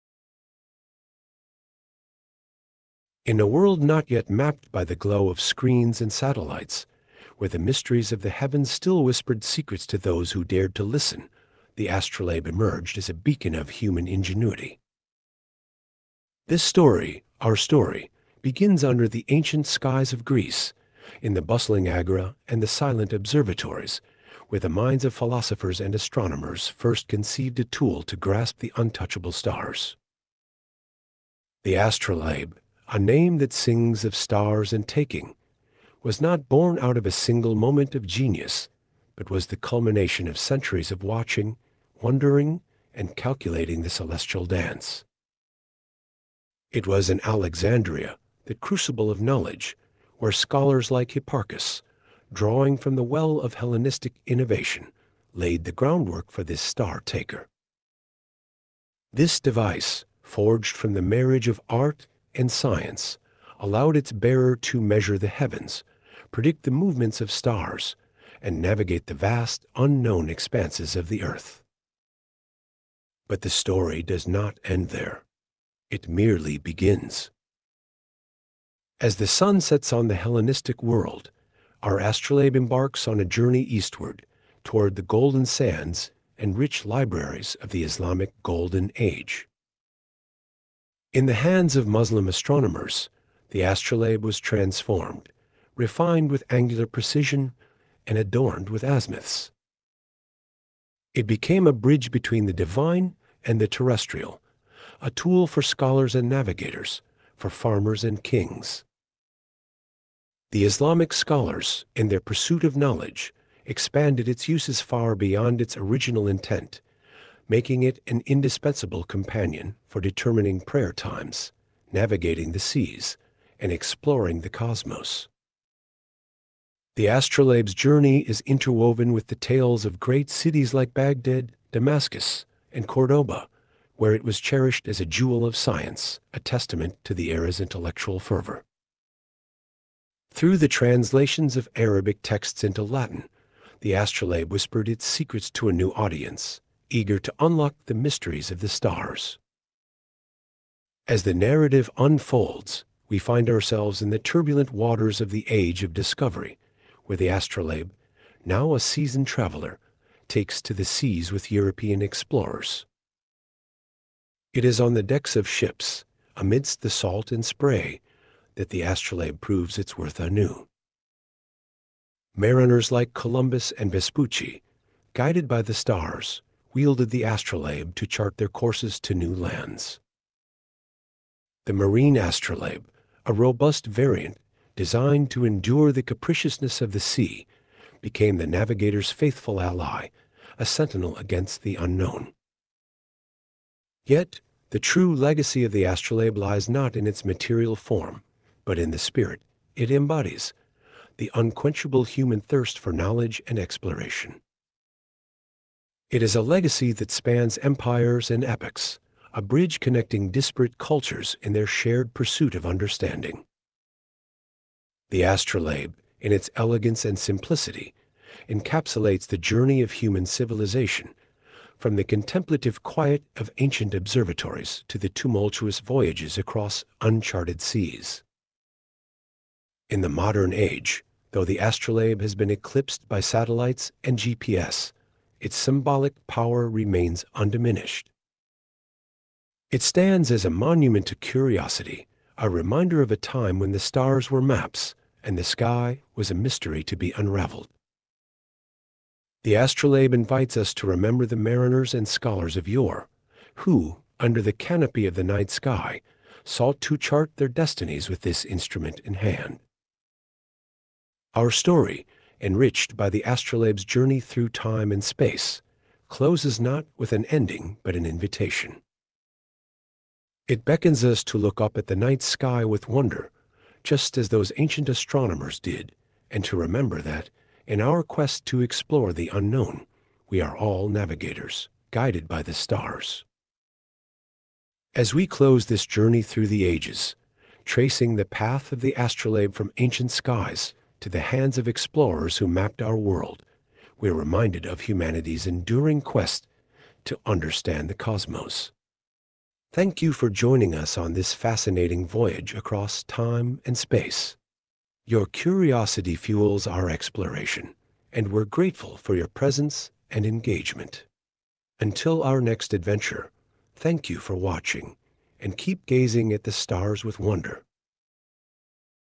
Astrolabe: Guiding Explorers from Ancient Agoras to the Age of Sail
Dive into the captivating story of the astrolabe, an ancient astronomical instrument that guided explorers, connected civilizations, and unlocked the mysteries of the stars. From its origins in the Greek agora to its refinement in the Islamic Golden Age and its role in the Age of Discovery, discover how the astrolabe shaped our understanding of the world and the cosmos. Join us as we explore the legacy of this remarkable tool across cultures and epochs, celebrating the human spirit of exploration and curiosity. Thank you for joining us on this journey through time and space.